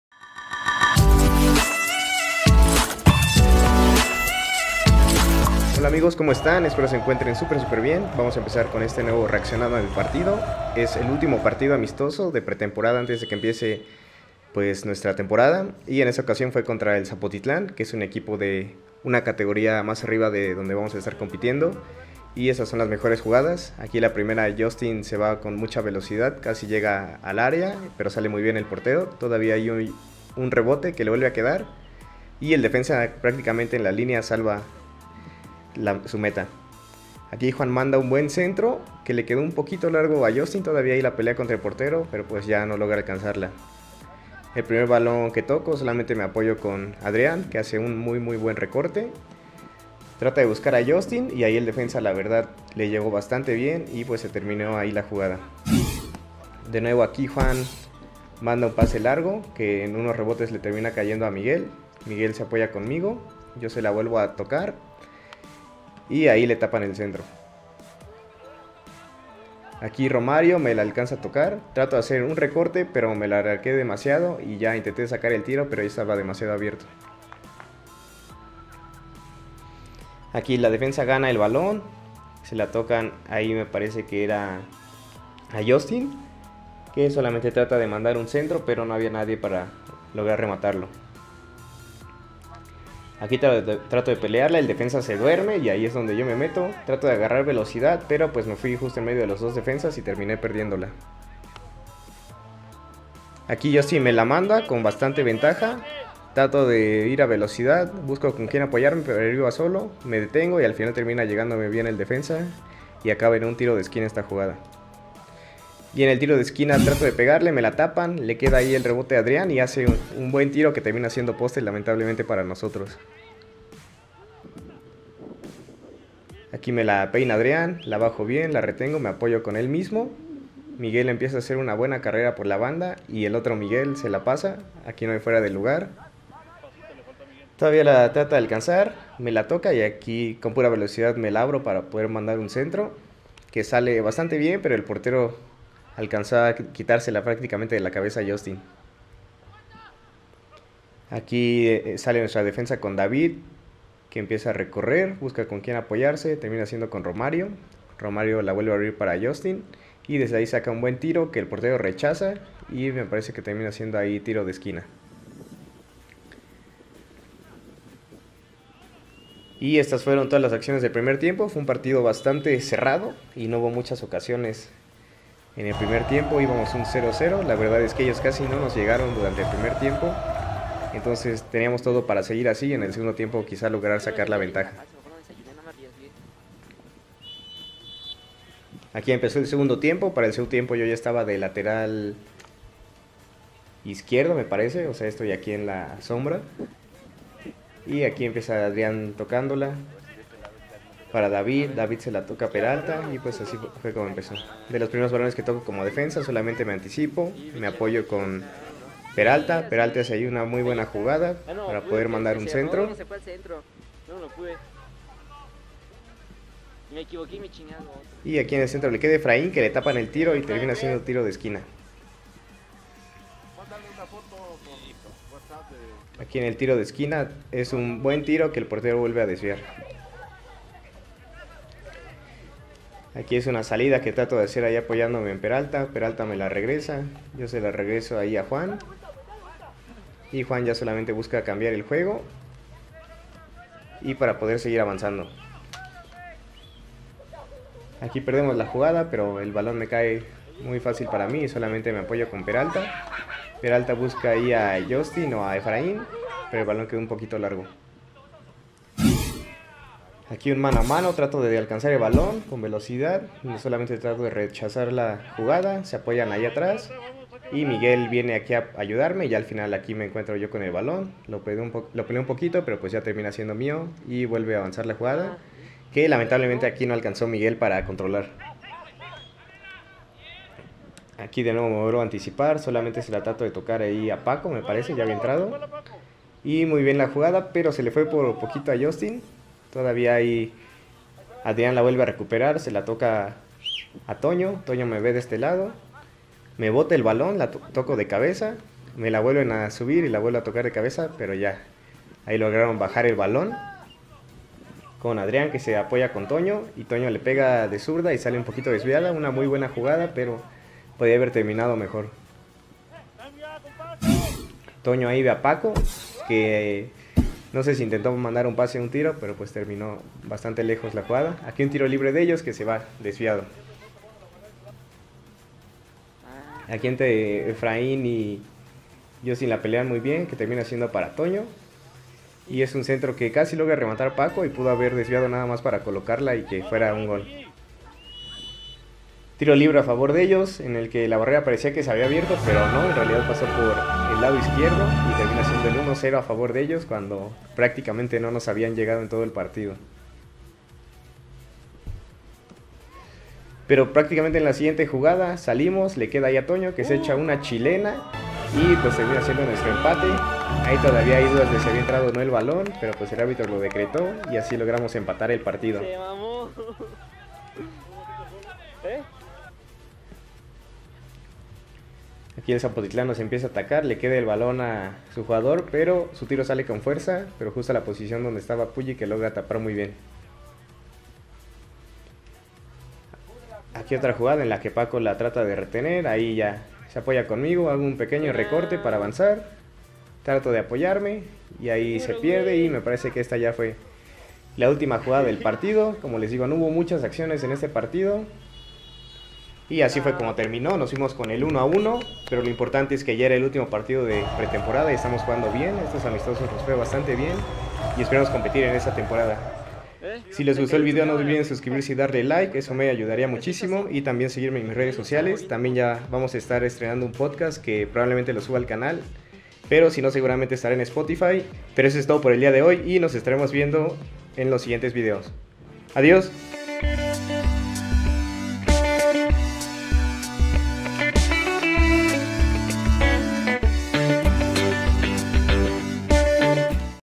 REACCIONANDO A MI PARTIDO.🥅 Se termina la PRETEMPORADA #12 |De Volea! ⚽️
Nuevo REACCIONANDO A MI PARTIDO con el ultimo juego de preparación antes de empezar la nueva temporada y empezar la competencia!

Donaciones directas para el canal!